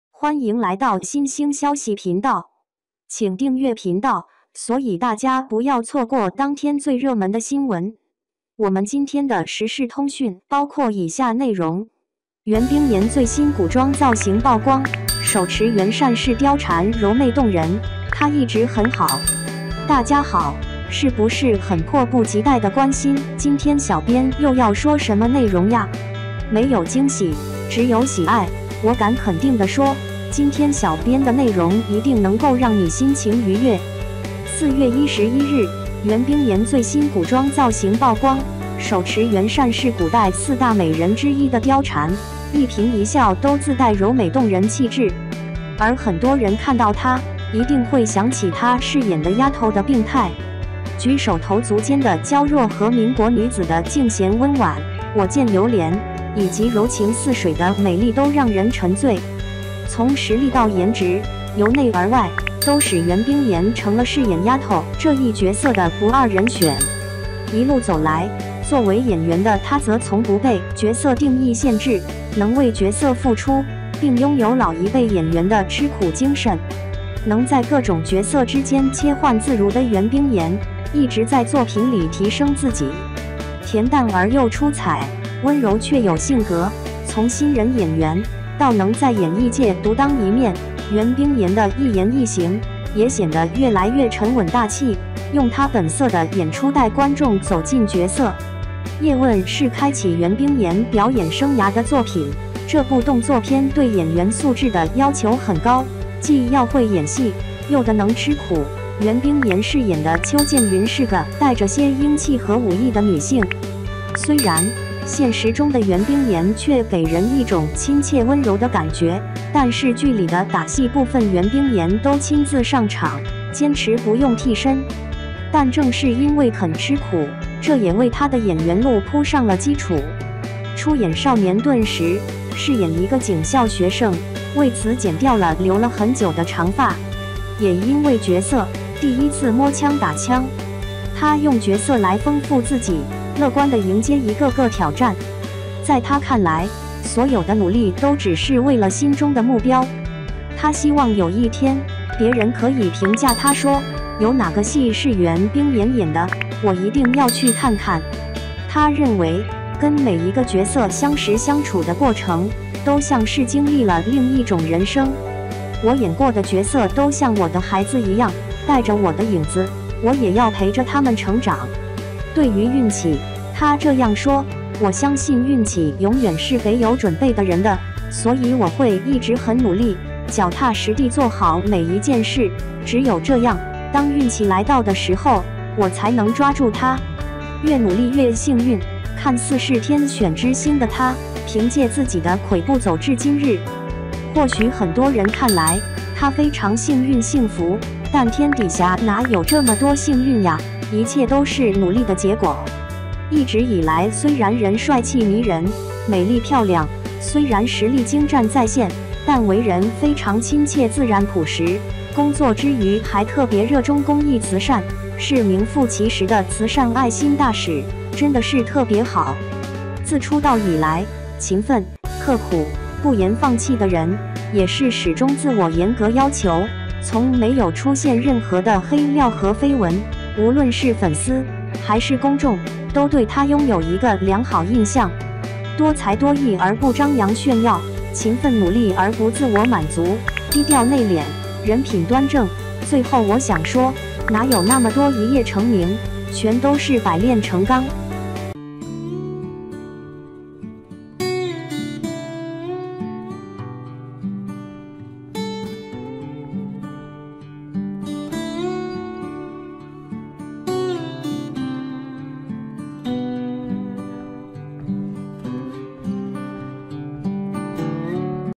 袁冰妍最新古装造型曝光，手持圆扇饰貂蝉柔媚动人，她一直很好
袁冰妍最新古装造型曝光，手持圆扇饰貂蝉柔媚动人，她一直很好 | 新星消息